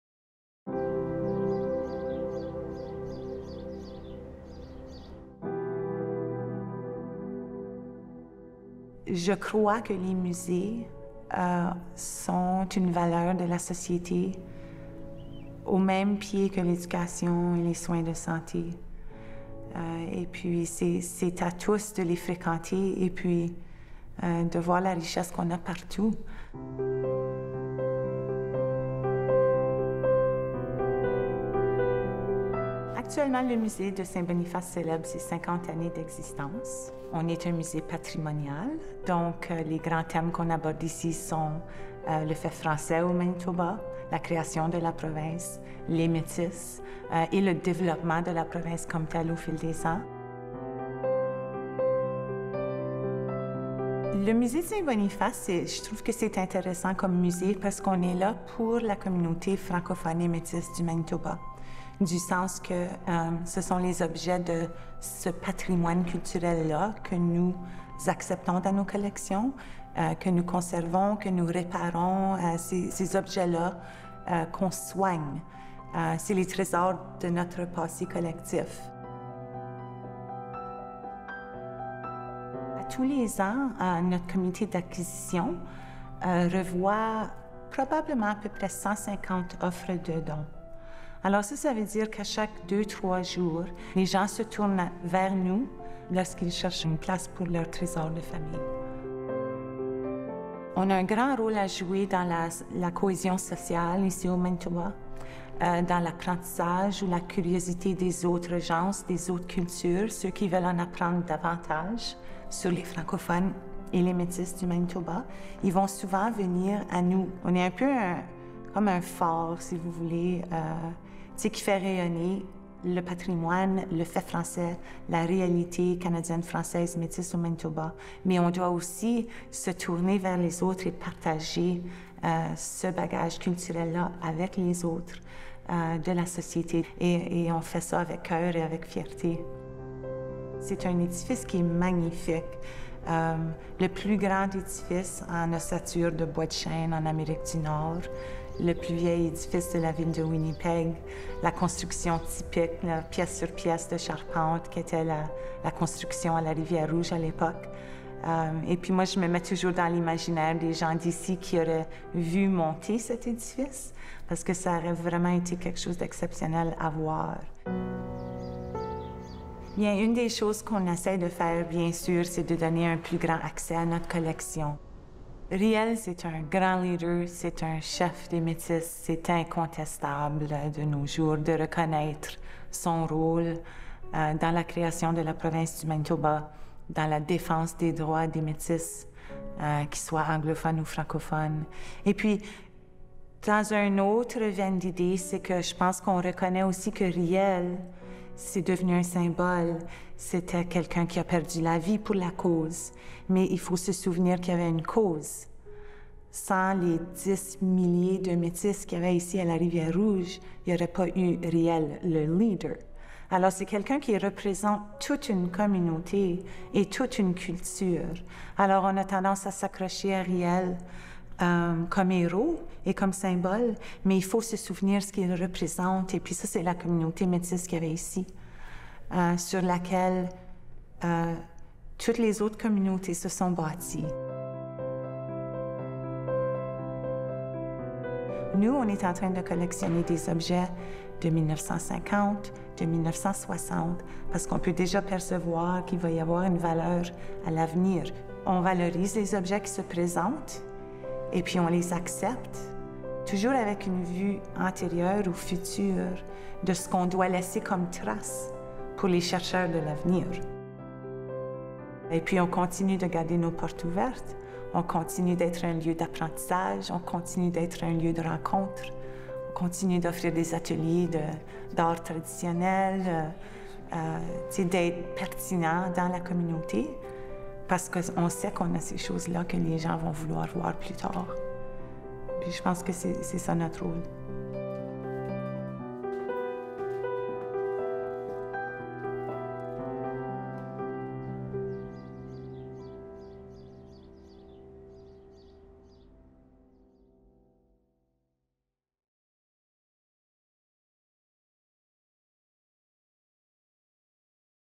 Le musée de Saint-Boniface
Logé dans l’ancien couvent des Sœurs Grises à Winnipeg, le musée de Saint-Boniface est consacré à la culture des francophones et des Métis du Manitoba. Vania Gagnon, directrice du musée, nous présente l’institution et quelques-uns des trésors qu’elle conserve.